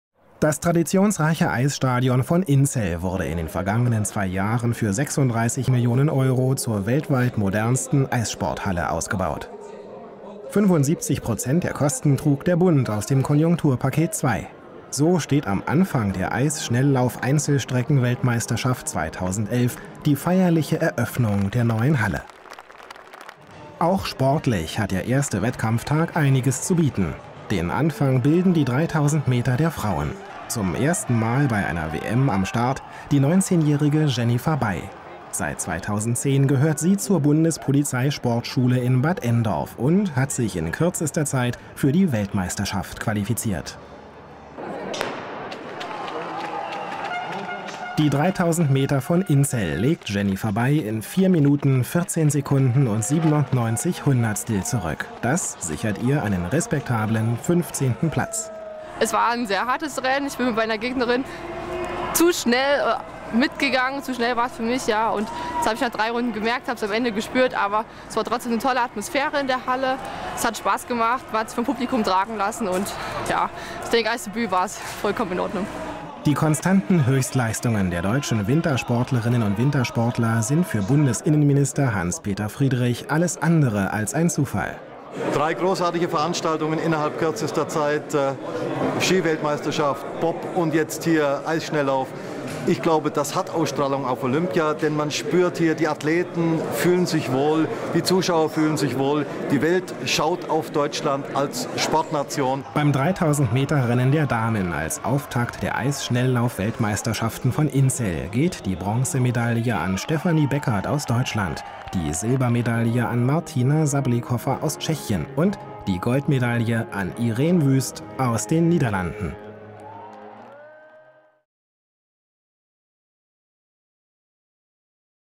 Eisschnelllauf-WM Inzell
Eindrücke der WM im Eisschnelllauf-WM 2011 in Inzell